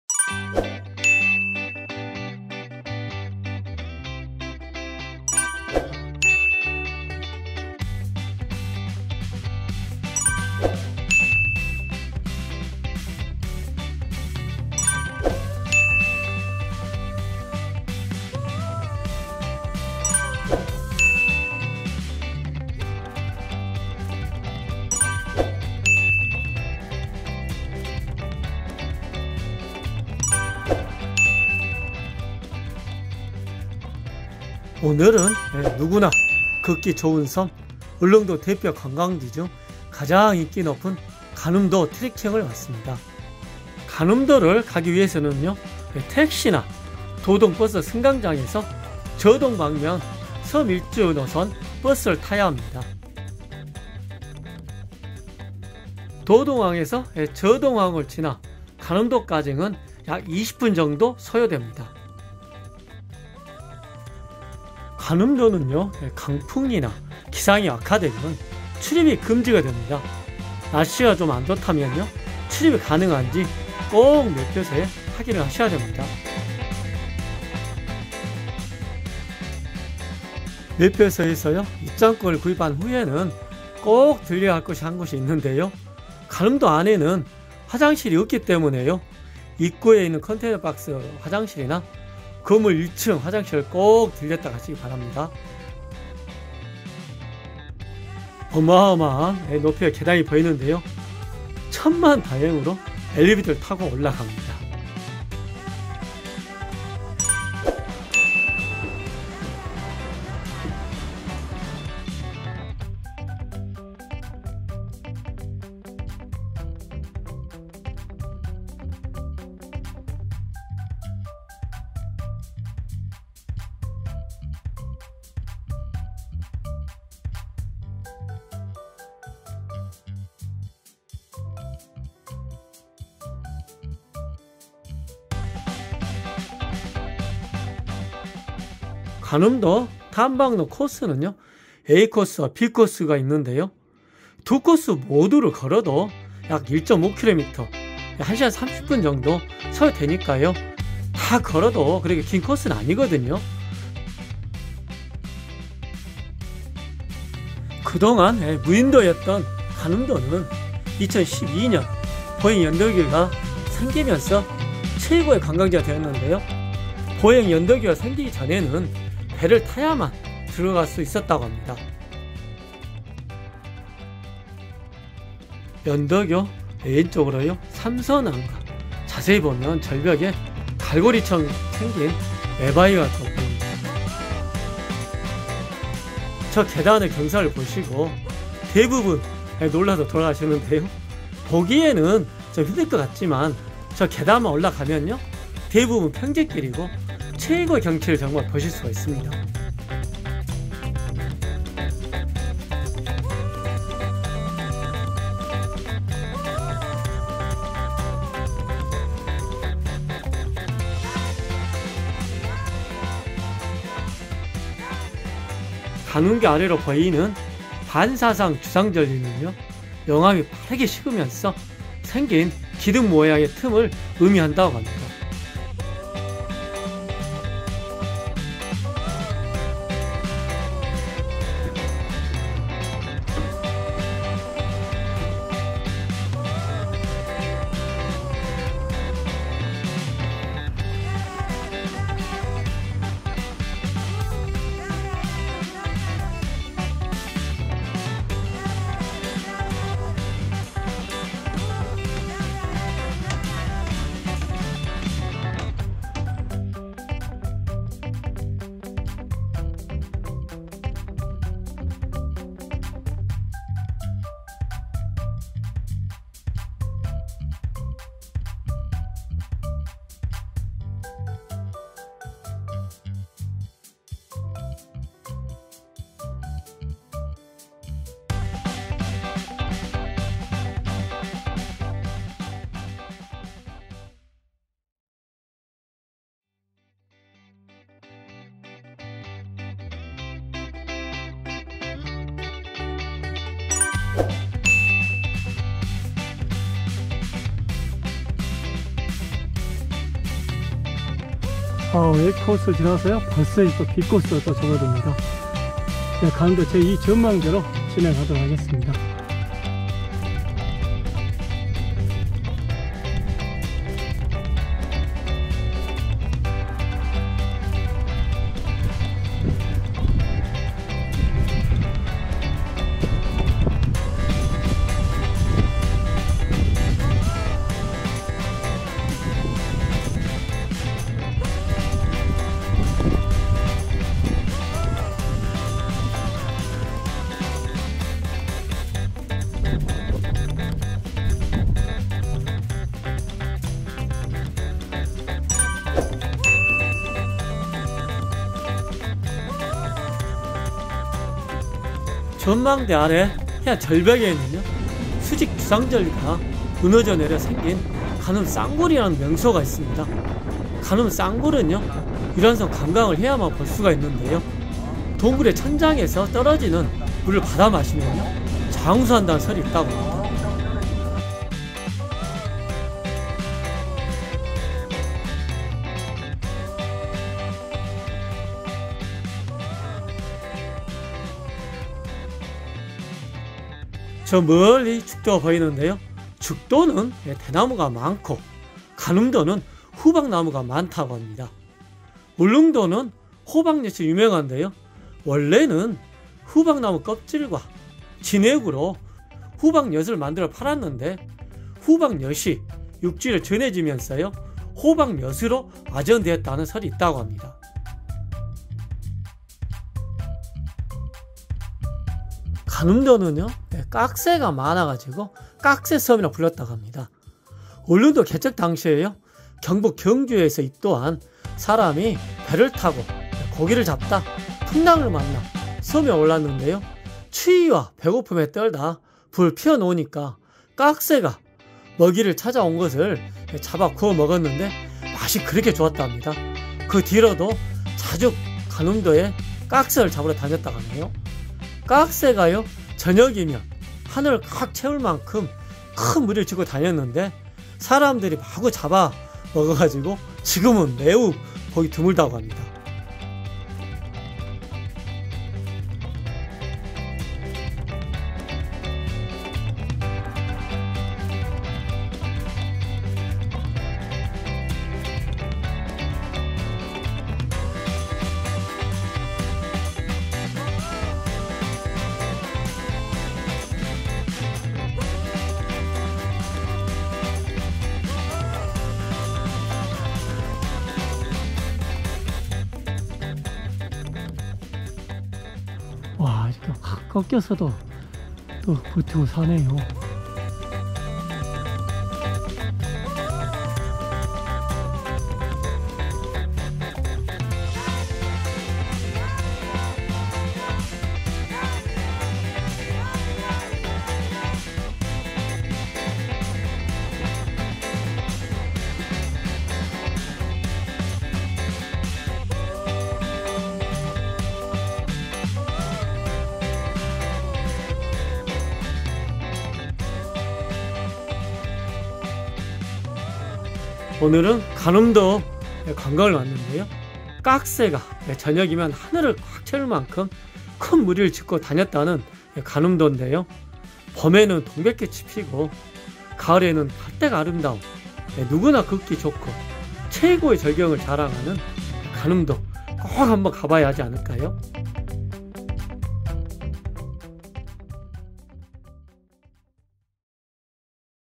울릉도관음도,울릉도대표관광지,누구나걷기좋은곳,깍새의고향.
깍새가 저녁이면 하늘을 꽉 채울만큼 큰무리를 짓고 다녔다던 관음도,
봄에는 동백꽂이 피고~
가을에는 억새가 아름다운 ~
누구나 걷기좋고 전망대에 서면 최고의 절경을 감상할수 있습니다

코스:매표소 ~연도교~전망대 2.3.1~매표소.
(원점회귀/약1.5km/1시간30분소요)